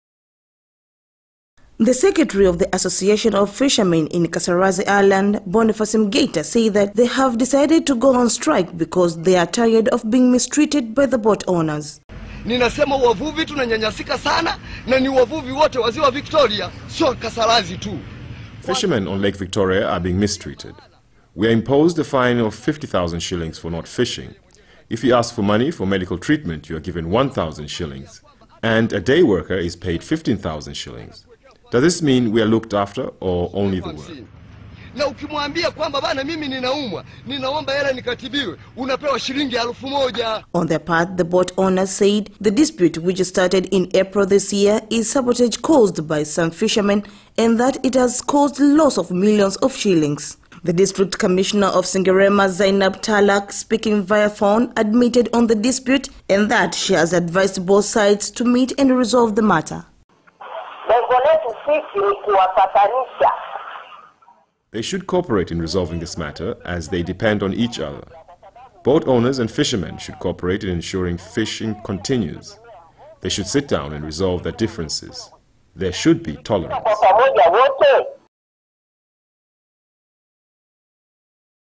More than 1,000 fishermen in Mwanza region strike
More than one thousand fishermen from Kasarazi island in Sengerema district in Mwanza region have gone on a strike for four consecutive days opposing harassment from boat owners.